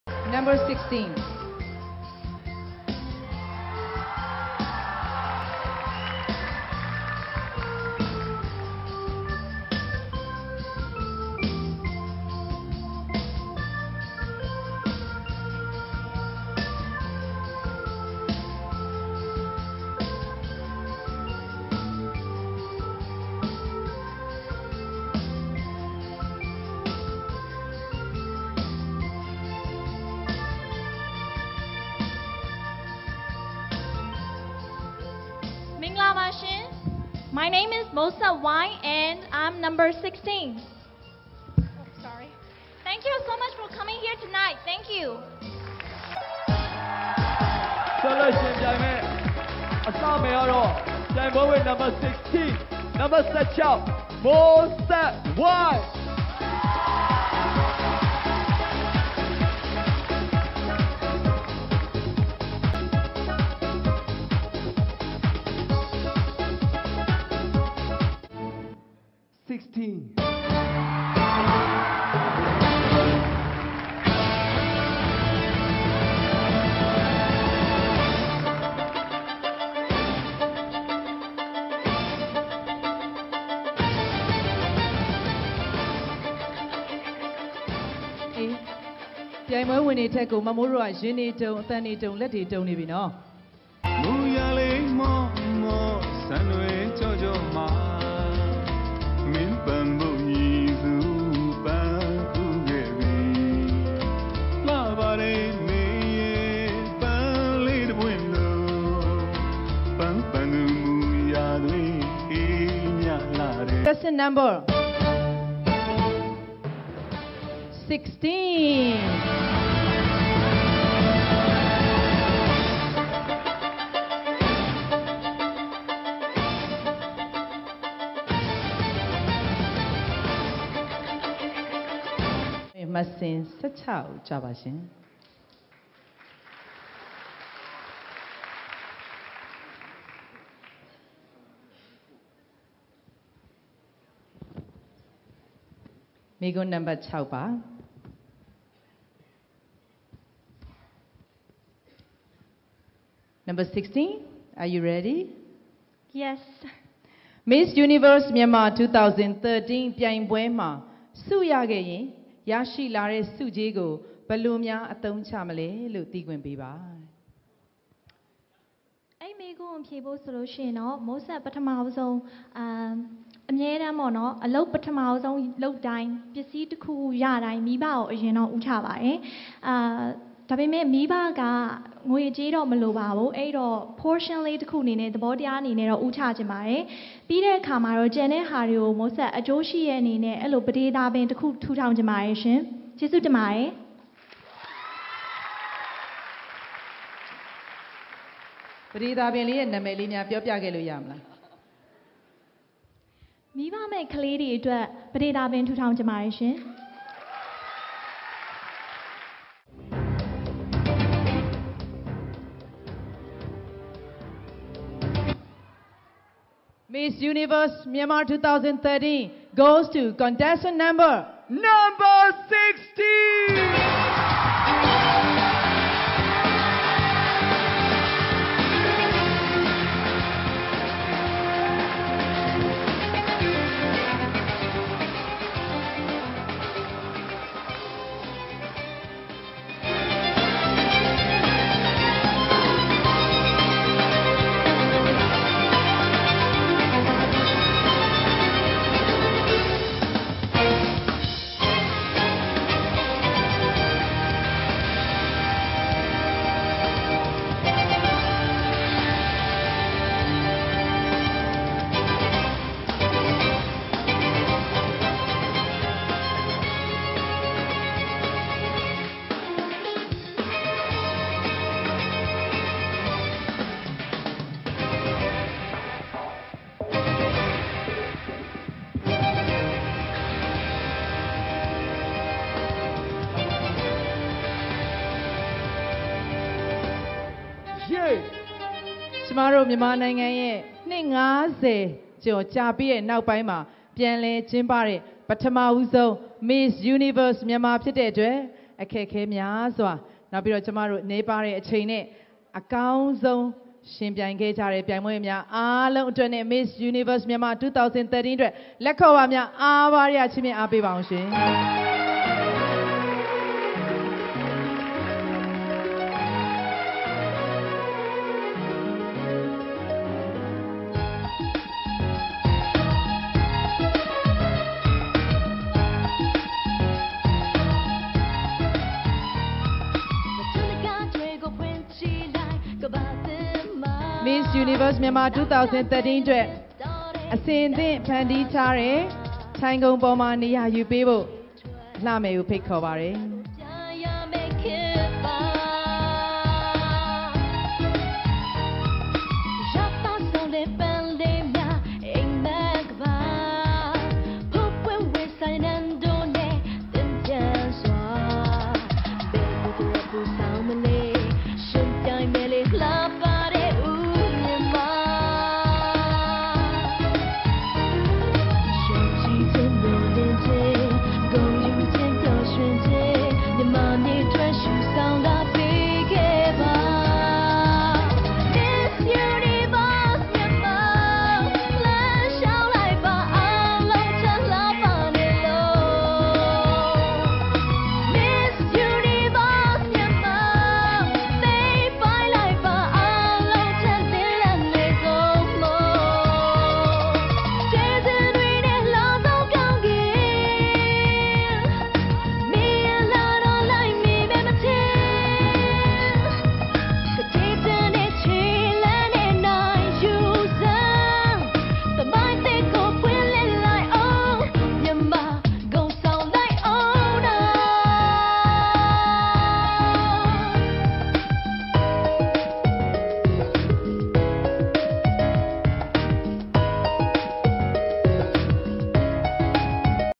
Miss Universe Myanmar 2013 Moe Sett Wine
Moe Sett Wine competed in Miss Universe Myanmar 2013 which was held on October 3, 2013 at the National Theatre in Yangon .The competition was organized by the Hello Madam Media Group . The competition was held for the first time since 1961.She won Miss Perfect Skin and Miss Famous titles,voted by fans through the Internet voting system and SMS messages.  She become the winner of Miss Universe Myanmar 2013 after the competition .She was the first woman to Represent the country in the competition since 1961.